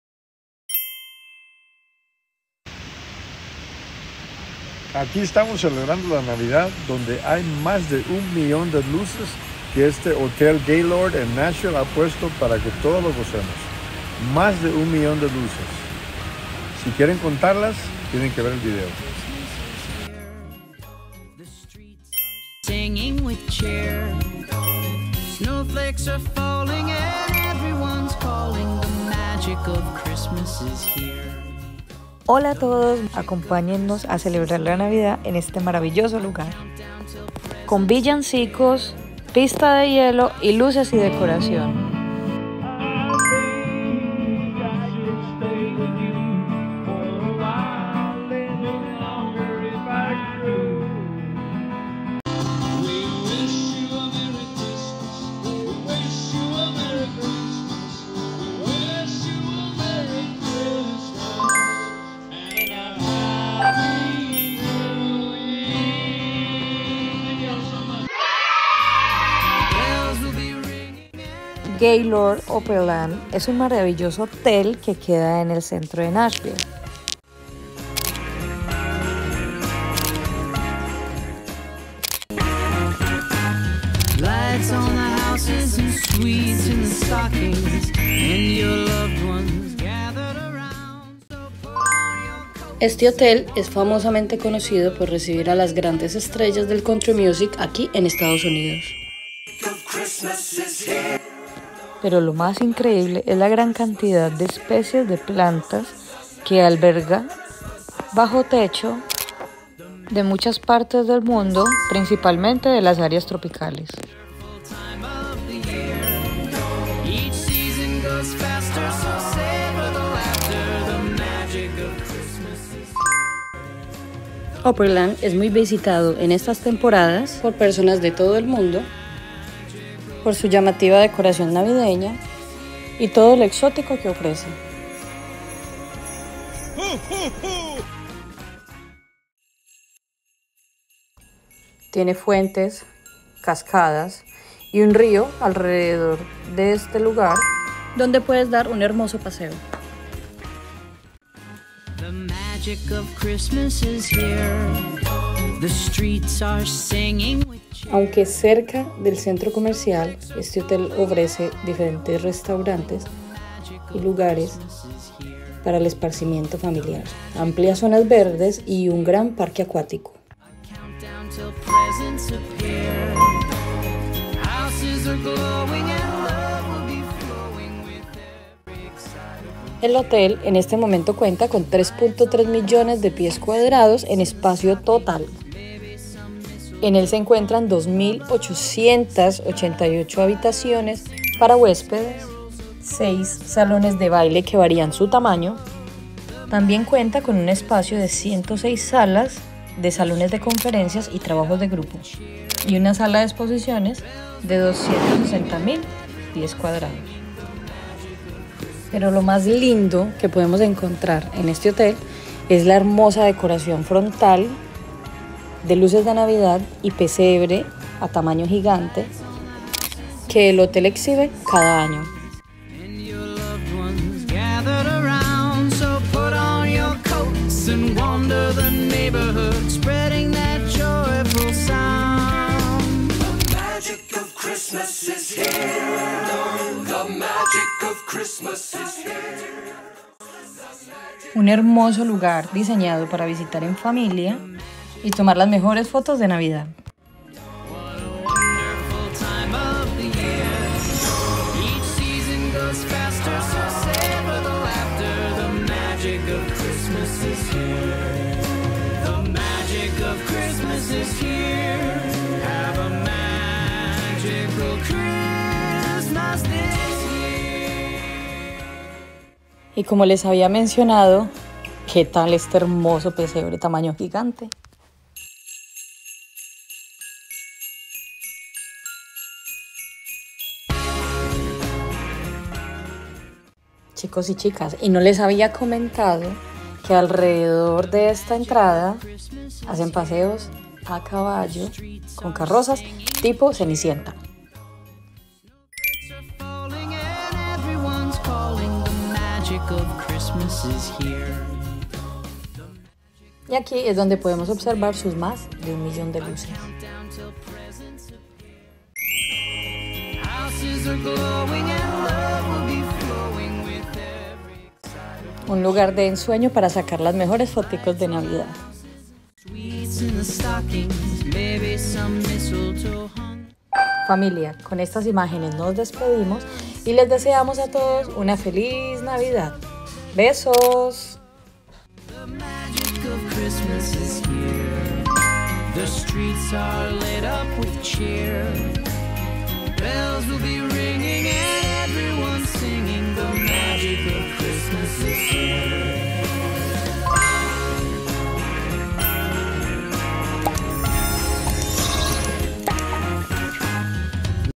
Como es una Navidad en Estados Unidos /Gaylord Opryland
Acompáñanos a recorrer uno de los hoteles más Grandes en Estados Unidos,  el hotel Gaylord Opryland en Nashville Tennessee, un hotel bajo techo con mas de 20 hectáreas de plantas tropicales durante todo el
año y además
una maravillosa decoración de Navidad un lugar para disfrutar en familia